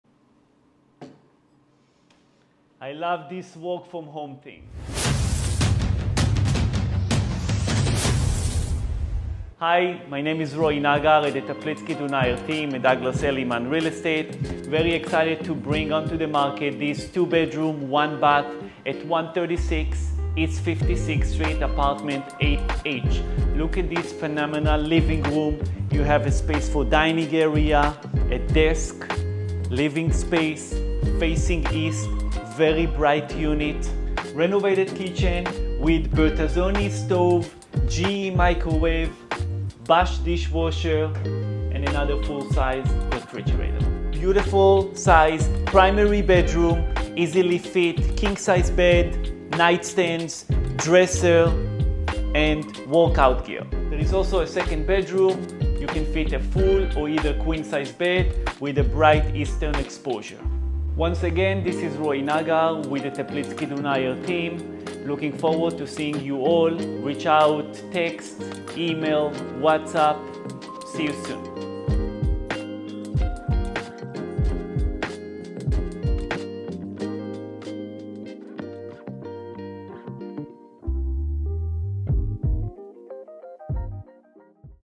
136 East 56th Street | Roi Nagar
Inviting Two-Bedroom Apartment in Prime Midtown Manhattan Location

Enter this comfortable world of urban living with this inviting two-bedroom apartment located in midtown Manhattan. Greet the morning with the soft warmth of east-facing sunlight, infusing the space with a calming energy. Spanning around 1000 sq ft, this apartment provides an ample canvas for relaxation and cherished moments.

Modern convenience meets style with the inclusion of an in-unit Bosch washer and dryer, ensuring everyday ease. The kitchen, tastefully renovated, boasts a Bertazzoni stove from Italy and a Bosch dishwasher, enhancing both functionality and aesthetics.

The building thoughtfully offers practical amenities, including a full-time doorman, a live-in superintendent, a basement laundry room, a bicycle rack, and a parking garage. Commuting becomes a breeze with the nearby 6,4,5 subway lines, and the 57th Street crosstown bus.

The cooperative’s policies add to the appeal, allowing for pied-a-terre, sublets, gifting, and co-purchasing, providing you with the flexibility to tailor your living situation. In close proximity, the convenience of a Whole Foods Market is complemented by an array of dining options, including renowned restaurants like Club A Steakhouse, The Lobster Club and Mr. Chow.

For a closer look at this inviting apartment, don’t hesitate to reach out and schedule a private viewing. Experience the true essence of Manhattan living in a space that effortlessly blends comfort, convenience, and culinary delights.